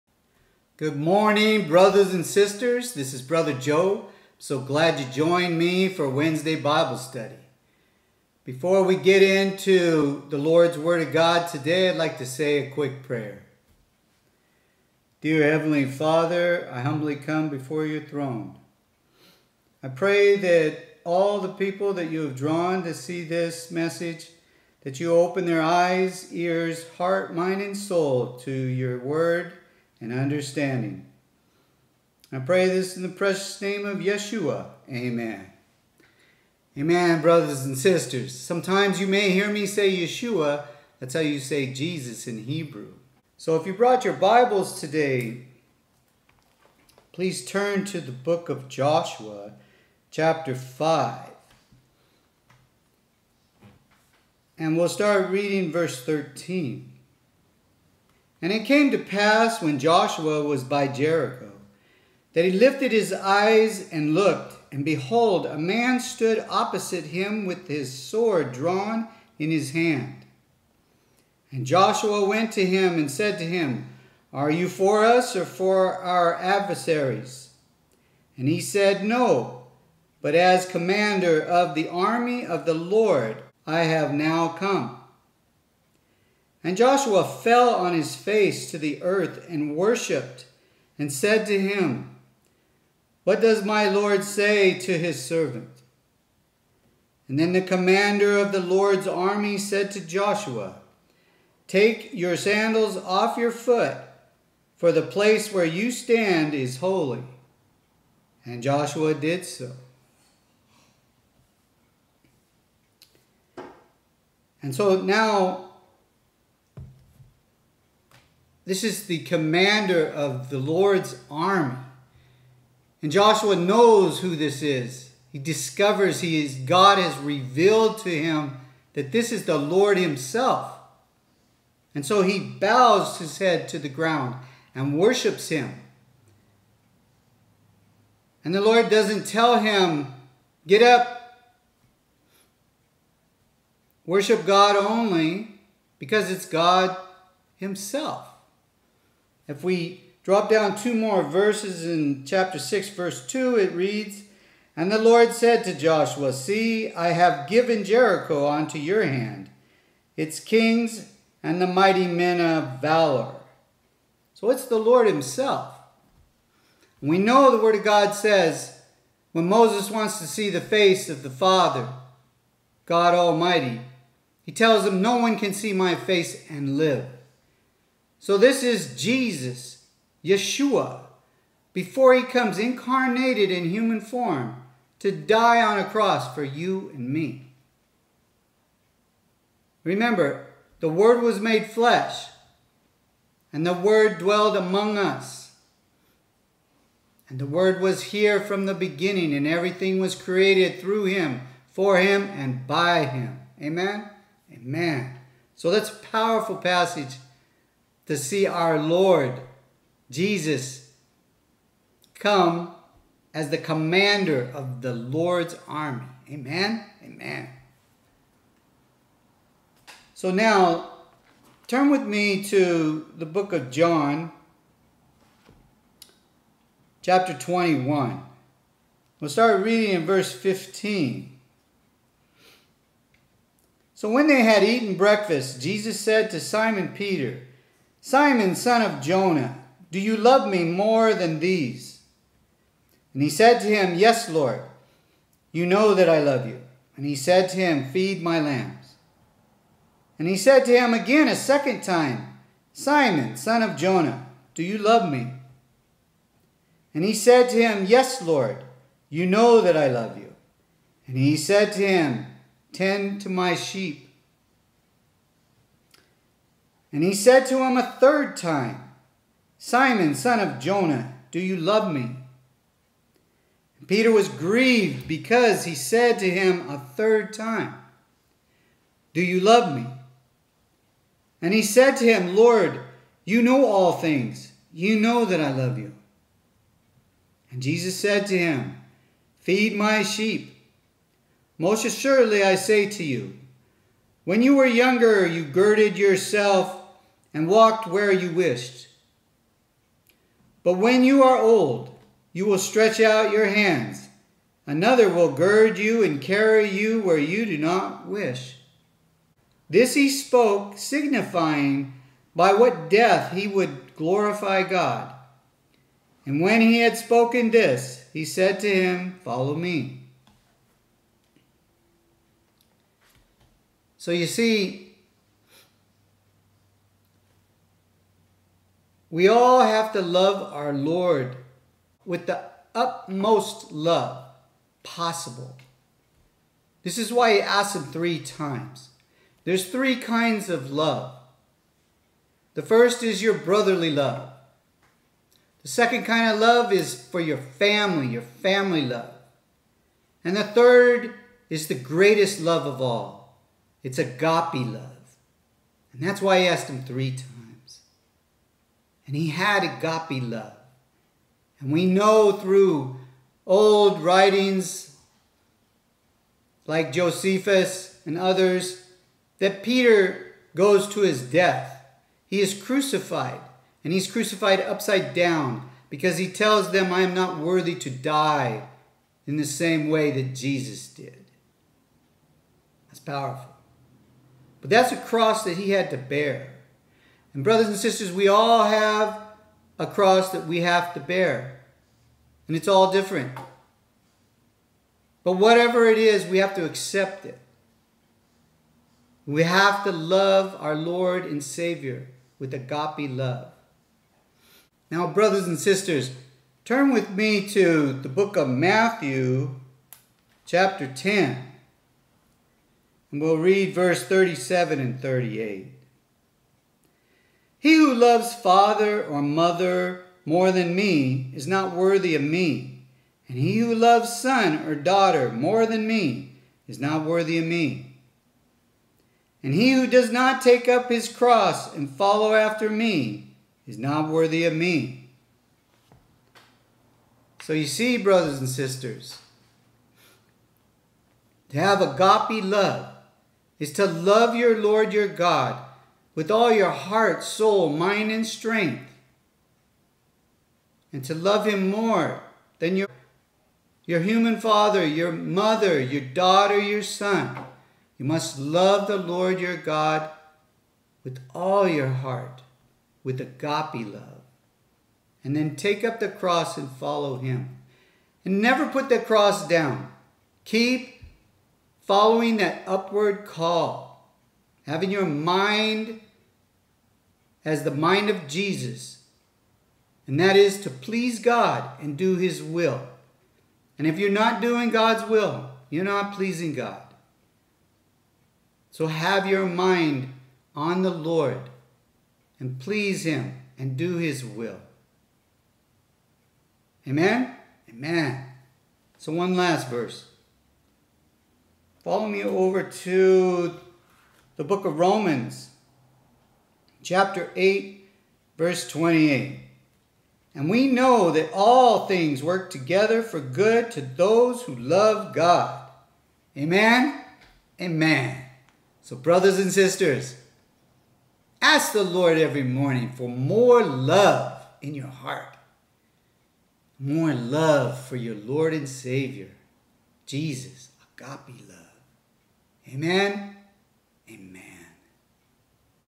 Agape Love@JustJoeNoTitle
Brother Joe, with the guidance and direction of the Holy Spirit, explains how we need to have Agape Love for our Lord and Savior.
Scriptures used:
Joshua 5:13-15;  6:2;
John 21:17-19;
Matthew 10:37-38;
Romans 8:28.